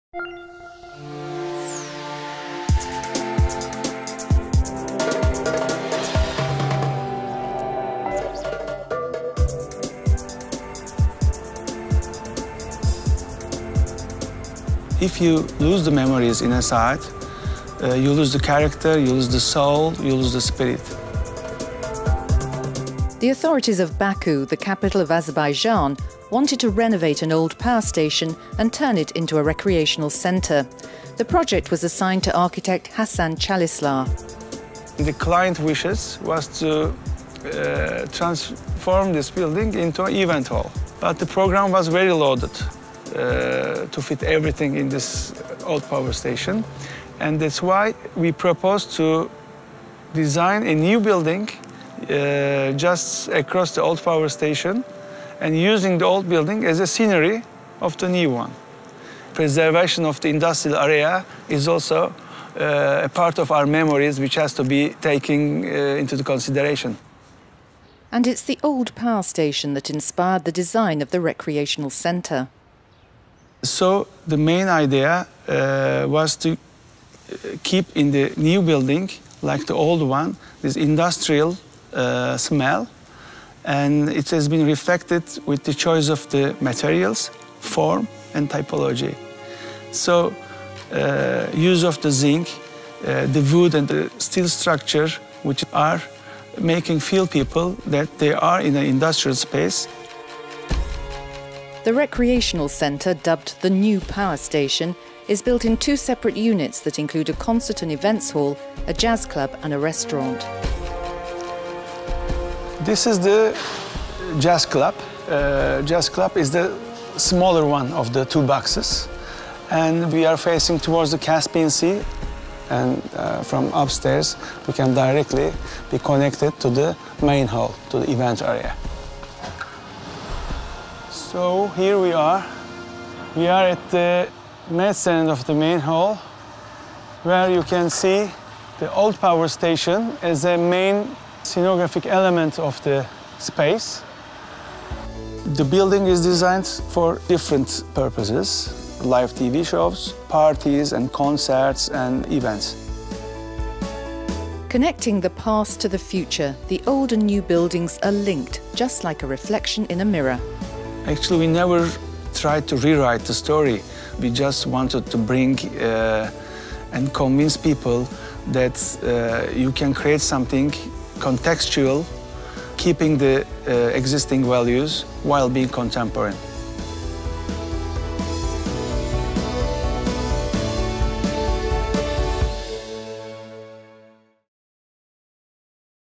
New Power Station - Azerbaijan
As part of the regeneration of an old industrial area, a new building echoes a recently restored power station beside it.